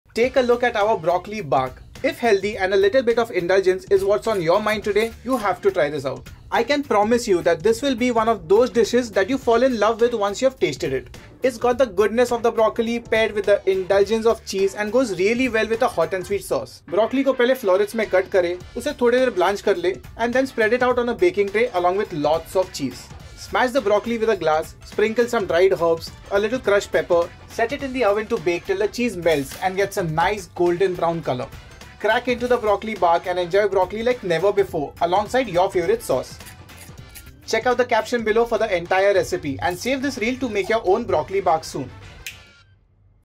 Crispy Broccoli Bark | Cheesy Recipe
"Introducing our Broccoli Bark loaded with the indulgence of Parmesan. Perfect for all our cheese aficionados out there, try it now and elevate your cheese game! 🧀🥦
Ingredients:
Broccoli Florets
Grated parmesan Cheese
Pepper
Dried herbs
Sweet and sour Sauce to serve with